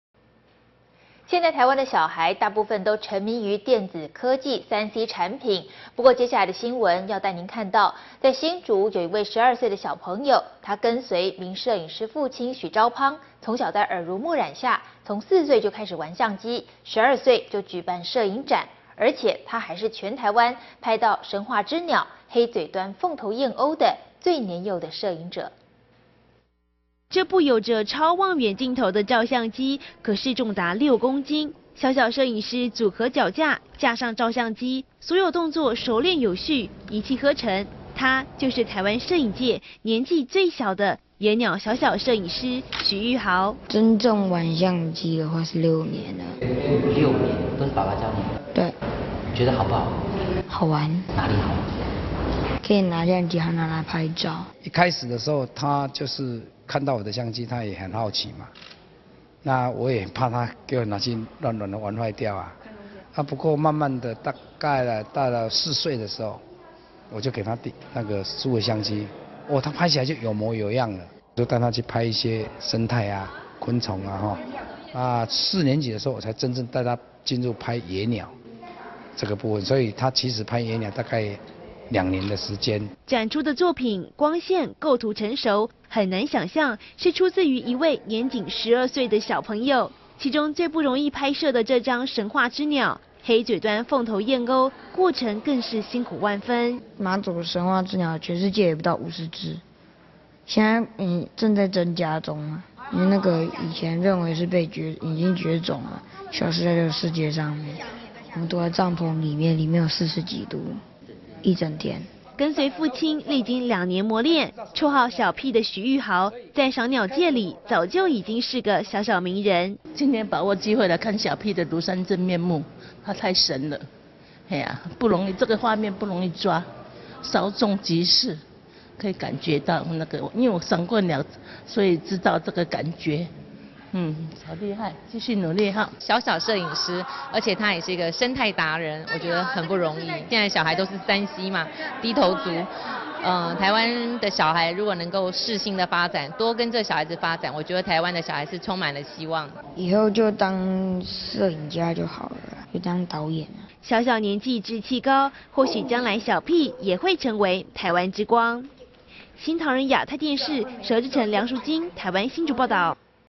12歲生態野鳥攝影師 小小年紀不簡單
【新唐人亞太台2012年5月29日訊】現在台灣的小孩，大部分都沈迷於電子科技3C產品。不過接下來的新聞要來帶您看到，在新竹有位12歲的小朋友，跟隨名攝影師父親許釗滂，從小在耳濡目染下，四歲開始玩相機，12歲就辦攝影展。而且他還是全臺灣拍到「神話之鳥」黑嘴端鳳頭燕鷗的最年幼攝影者。

這部有著超望遠鏡頭的照相機，可是重達6公斤，小小攝影師組合腳架，架上照相機，所有動作熟練有序，一氣呵成，他，就是台灣攝影界年紀最小的，野鳥小小攝影師許喻豪。

小小攝影師 許喻豪：「真正玩相機是六年。」
記者：(都是爸爸教你的嗎?)
小小攝影師 許喻豪：「對。」
記者：(覺得好不好玩?)
小小攝影師 許喻豪：「好玩。」
記者：(那裡好玩?)
小小攝影師 許喻豪：「可以拿相機，和它拍照。」

台灣知名風景攝影師 許釗滂：「一開始的時候他就看到我的相機，他也很好奇嘛，那我也怕他給我拿去亂玩玩壞掉啊，不過慢慢的大概4歲的時候，我就給他數位相機，喔，他拍起來就有模有樣了。都帶他去拍一些生態啊昆蟲，那4年級的時候我才真正帶他進入去拍野鳥，那他其實拍野鳥大概兩年的時間。」

展出的作品，光線構圖成熟，很難想像，是出自於一位年僅12歲的小朋友，其中最不容易拍攝的這張「神話之鳥」黑嘴端鳳頭燕鷗，過程更是辛苦萬分。

小小攝影師 許喻豪：「馬祖那個時候那隻鳥全世界也不到50隻，現在正在增加中啦，因為那個以前認為是已經絕種了，消失在這個世界上面。我們躲在帳篷裏面，裡面有40 幾度，一整天。」

跟隨父親，歷經2年磨練，綽號小鷿的許喻豪，在賞鳥界裡，早就已經是個小小名人。

民眾：「今天把握機會來看小鷿的廬山真面目，他太神了，不容易，這個畫面不容易抓，稍縱即逝可以感覺到這個，因為我賞過鳥所以知道這個感覺，嗯，很厲害繼續努力。」

中華大學校護 ：「小小攝影師而且他也是一個生態達人，我覺得很不容易。現在小孩都是3C嘛，低頭族，台灣的小孩能夠適性的發展，多跟著小孩的發展，我覺得台灣的小孩是 充滿了希望。」

小小攝影師 許喻豪：「以後就當攝影家就好了，就當導演。」

小小年紀志氣高，或許將來小鷿也會成為台灣之光。

新唐人亞太電視 佘志誠 梁淑菁 台灣新竹採訪報導

其他推薦:
《神韻》《SHEN YUN》